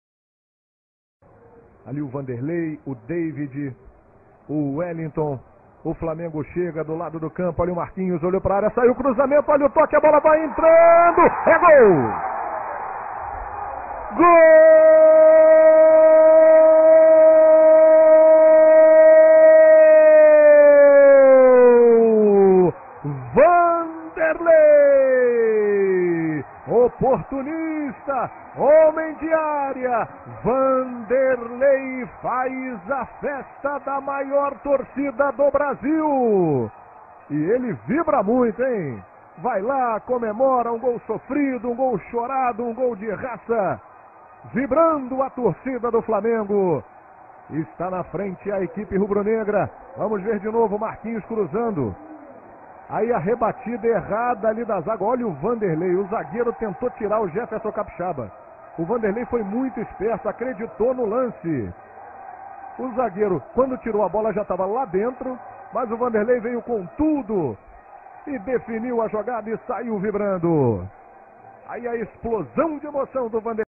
Wanderley [Flamengo 02 x 00 Americano - Carioca 26/01/2011
Primeiro dos dois gols em Macaé